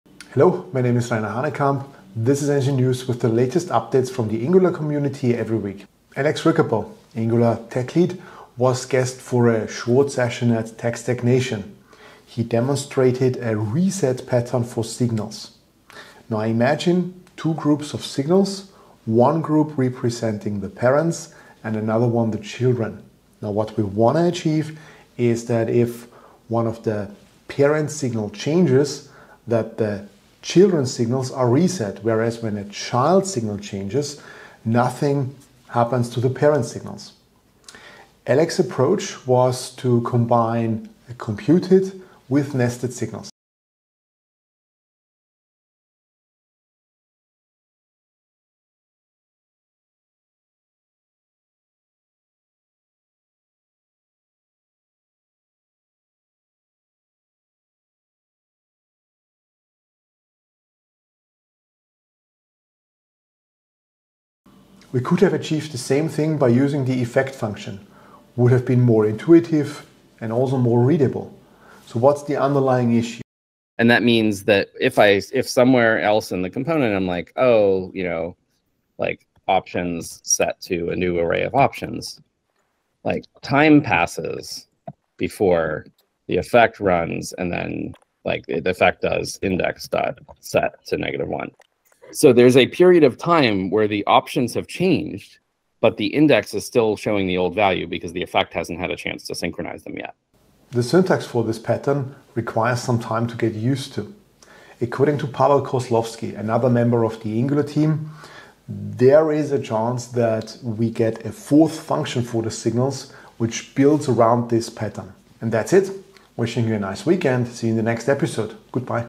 Episode 24/37: Reset Pattern for Signals
Alex Rickabaugh, the tech lead of the Angular framework, appeared at TechStackNation to showcase a reset pattern for Signals.

He demonstrated a scenario involving two groups of Signals—parents and children—where changes in the parent group reset the children, while changes in the children do not affect the parents.

Alex’s solution involved combining a `computed` Signal with nested Signals to achieve this behavior.

This approach is also useful in cases like `input` functions, where a read-only signal needs to be updated within a component but should still reflect any changes from the parent.

While an effect could achieve a similar result, computed signals offer synchronous updates, avoiding delays in signal synchronization caused by asynchronous effects.

According to Pawel Koszlowski, the Angular team is considering adding a new function for this pattern, potentially as soon as Angular 19.